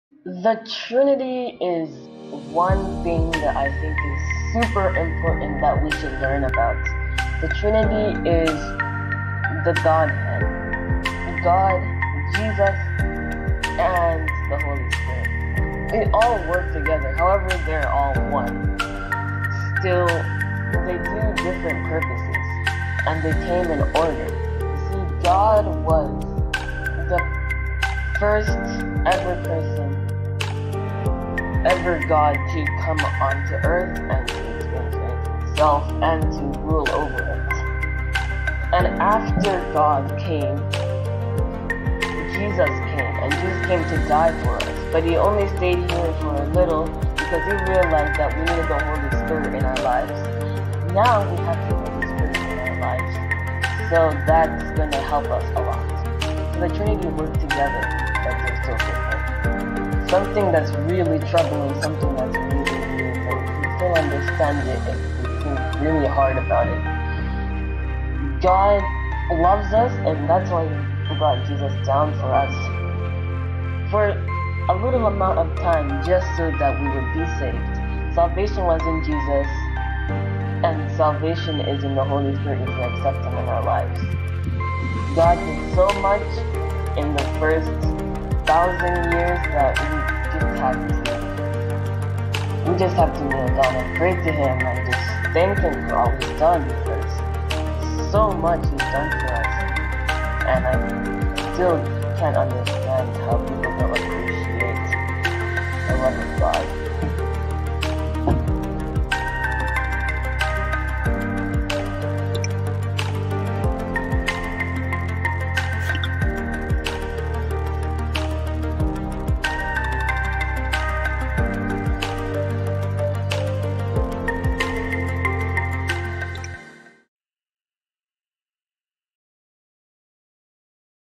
The Trinity
This video will explain to you what the Trinity is.

Lets make Lambs for Jesus